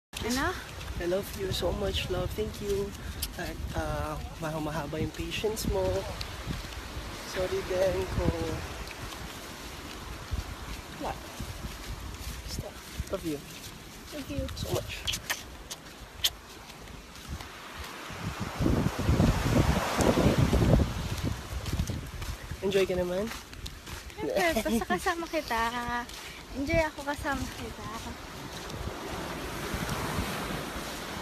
Franco Hernandez Death: GF shares last video with Hashtag Franco
Franco Hernandez' girlfriend Janica Nam Floresca has shared this video of them together taken just minutes before Franco's death.

Rest in Peace, Hashtag Franco.